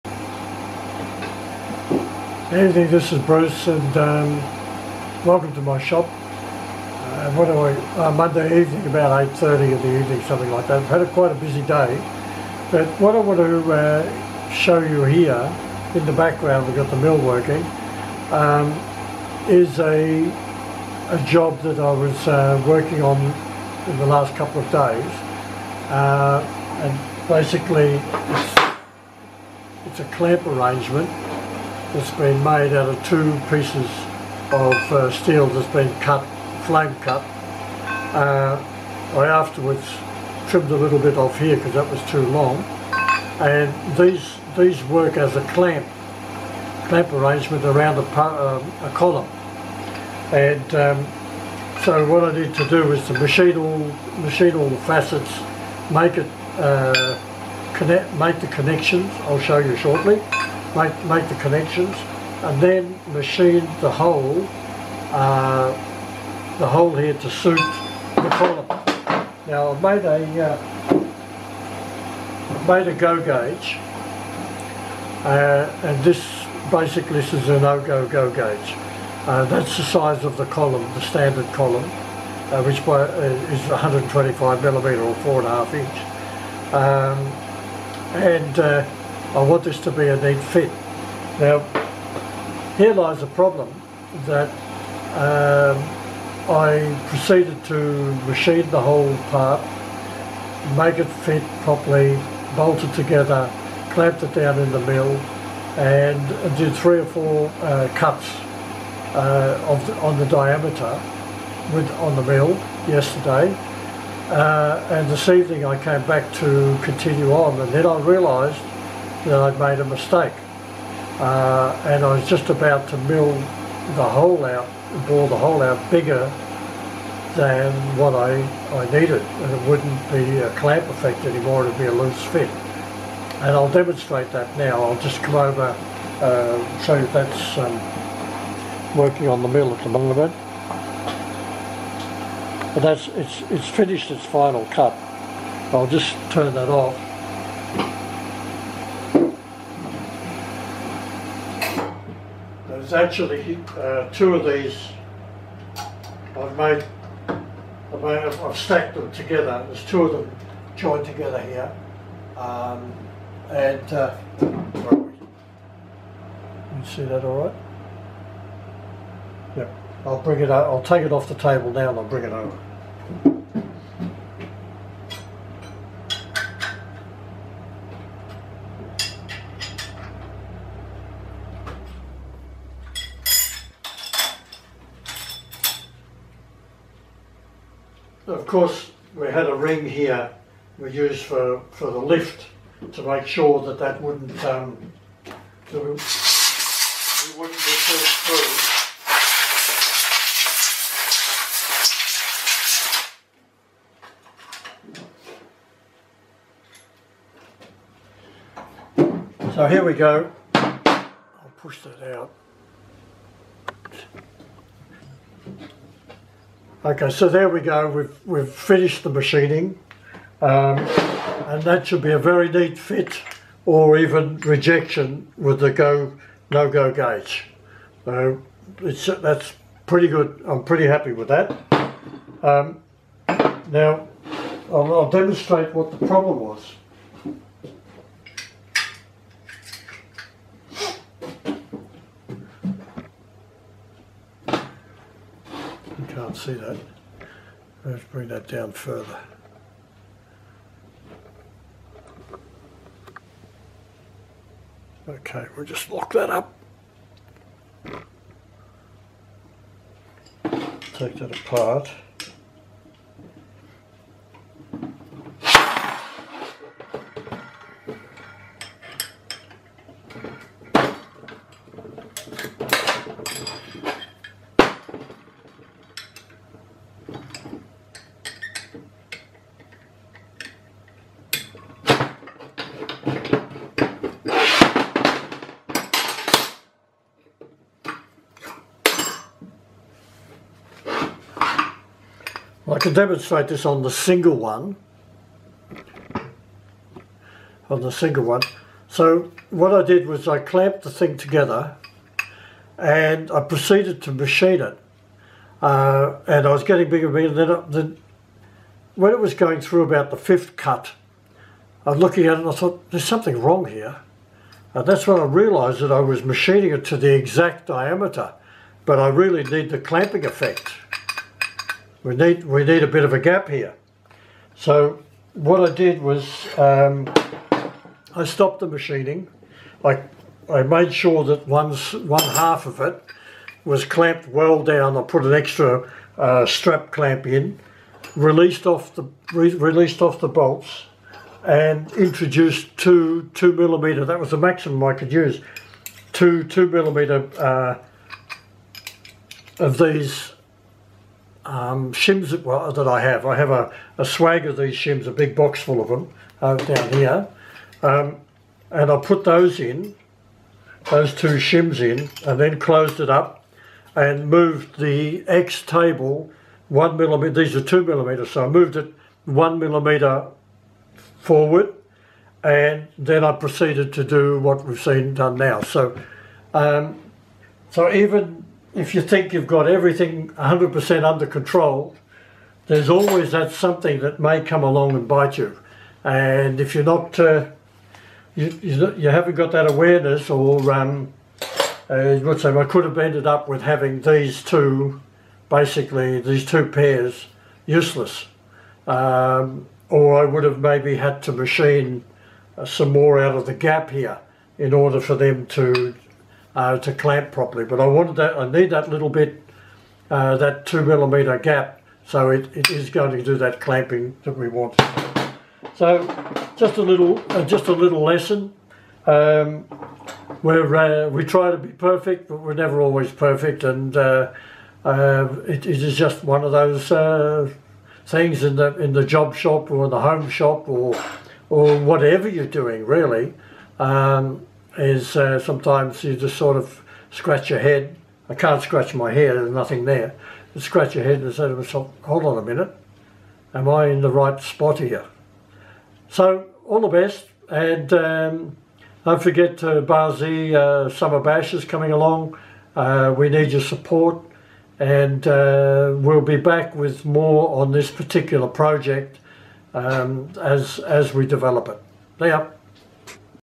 Machining Mistakes. How close can you get to the finish line before you realise UH OH!
Almost finished this boring job and realised the setup was not correct. Machining a two part clamp system for a project, that could of added more unwanted issues to the job. We all make mistakes or we hammer away without realising we may be creating a problem down the track. caught it on time.